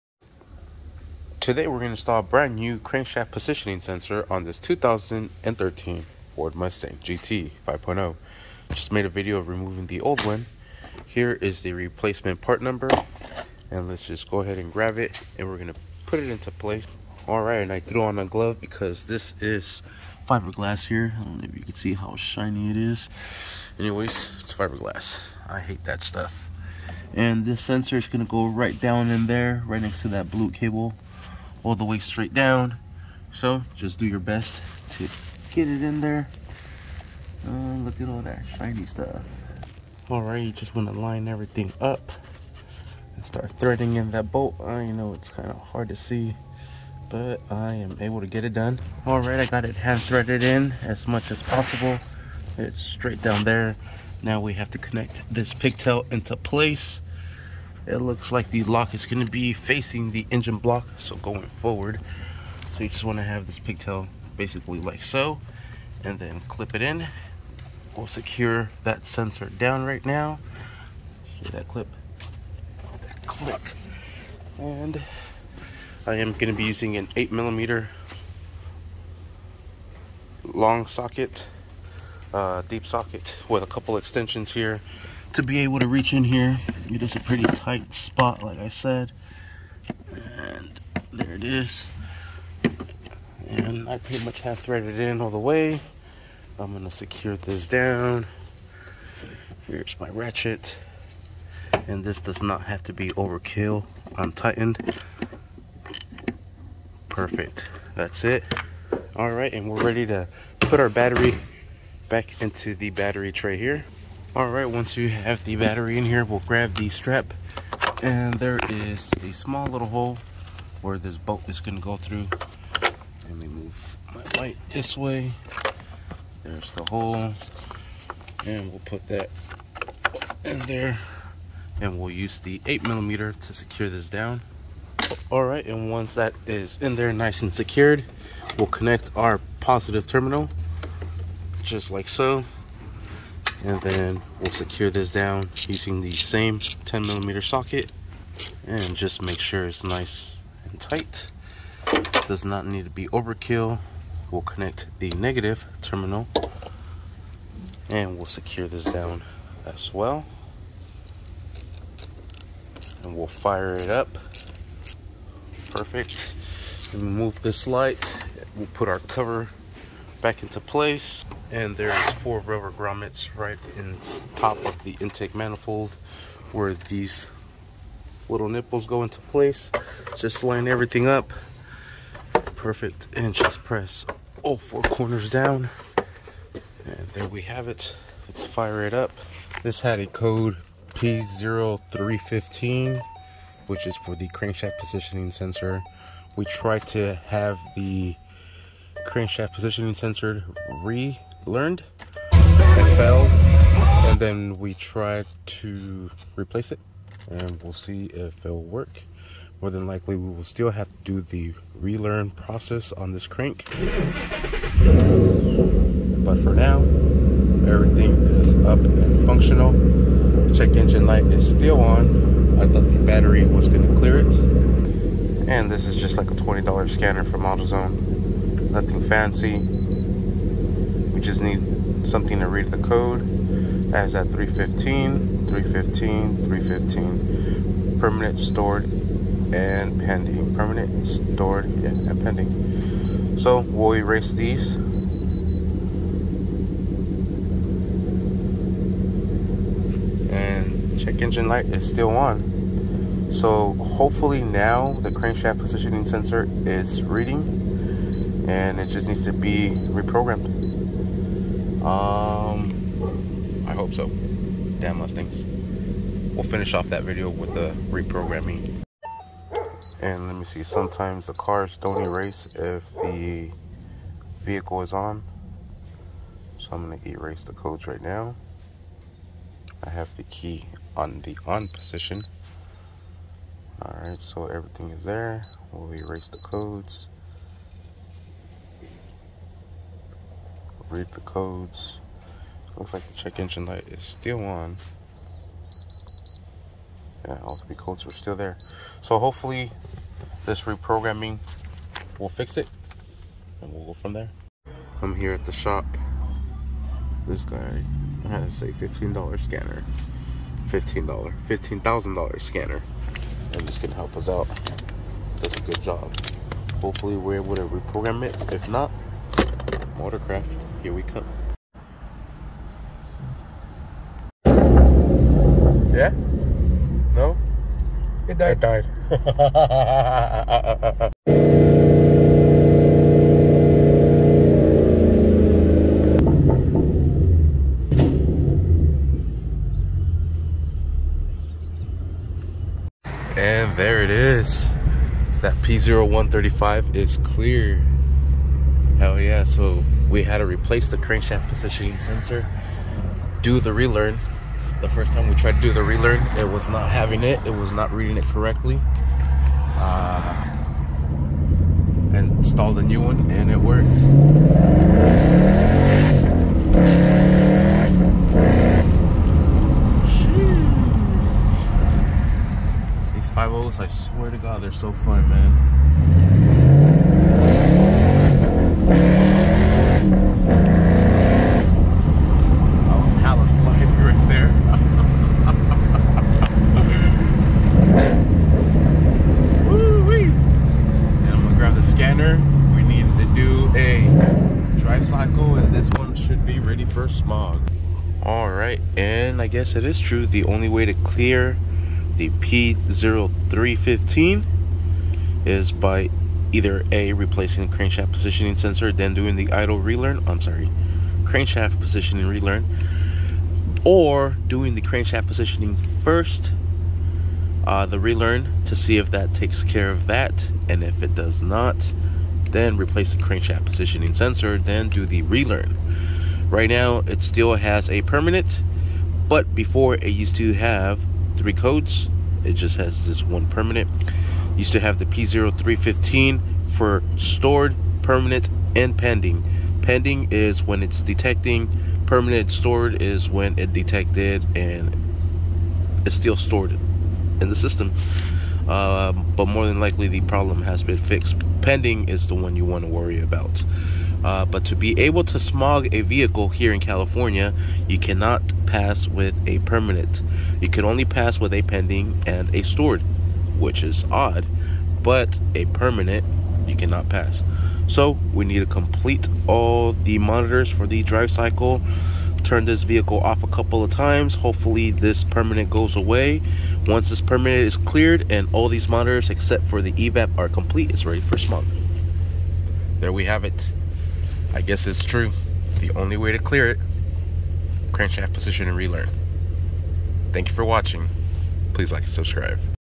How to: Install Crankshaft Positioning Sensor Mustang GT 5.0 (Recalibration is Required)
Thank you for all your Support! New content uploaded Daily from different Makes and Models.

THANK YOU FOR ALL YOUR TIME AND SUPPORT, MUCH APPRECIATED! - George Lopez